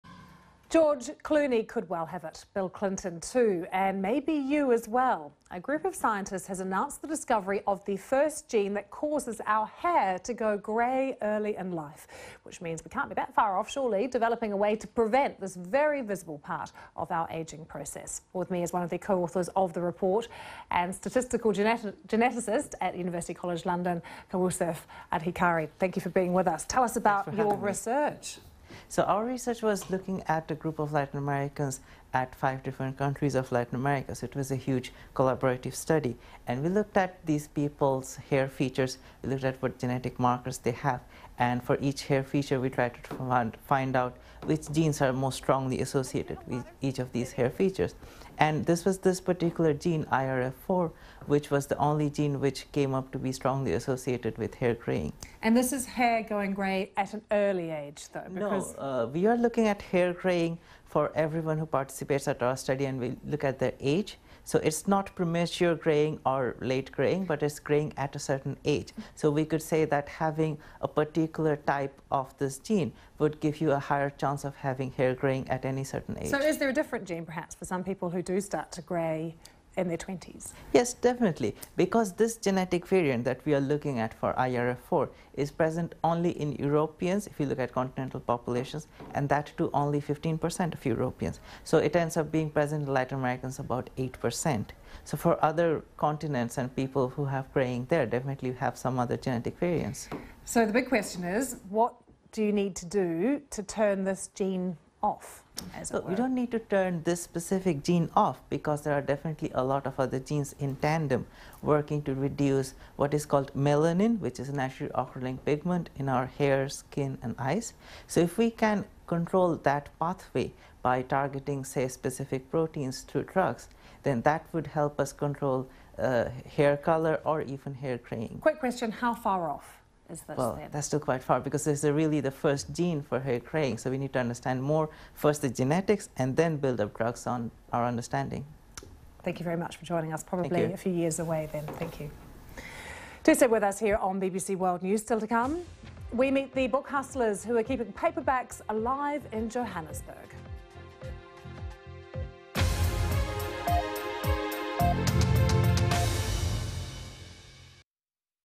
Grey hair and the IRF4 gene: Kaustubh Adhikari on BBC World News
Courtesy of BBC World News. Aired on 2nd March 2016.

Reference:
Adhikari, K. et al. A genome-wide association scan in admixed Latin Americans identifies loci influencing facial and scalp hair features. Nat. Commun. 7:10815 doi: 10.1038/ncomms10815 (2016).